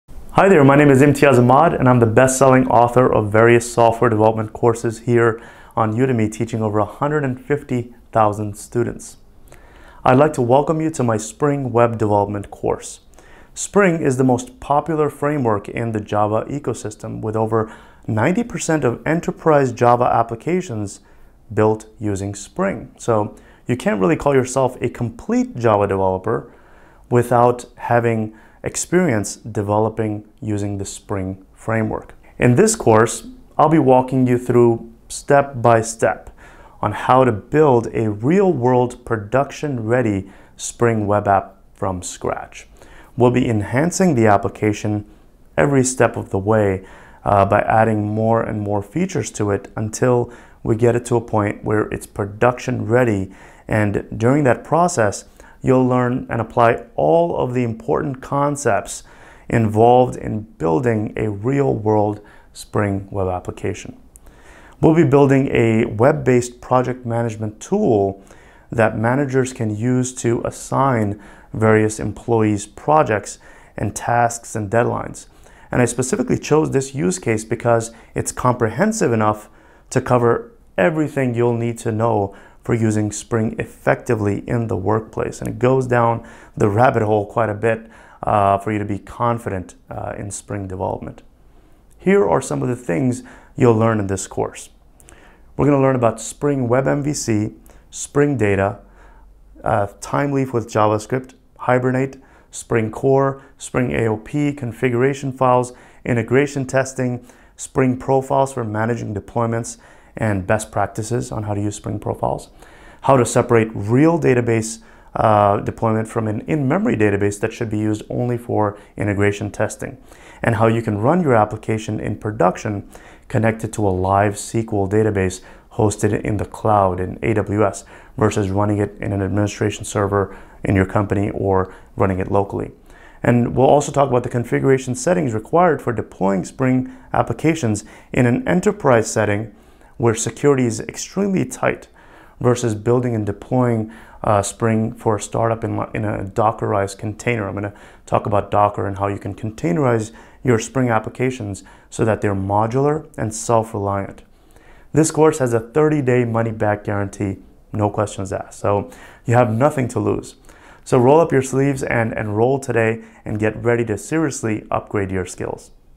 Spring Framework 5 Boot 2 Course
The Promo video for my Spring Framework Flagship course is available on Udemy and jobreadyprogrammer.com.

- Access all of my software development courses using the discount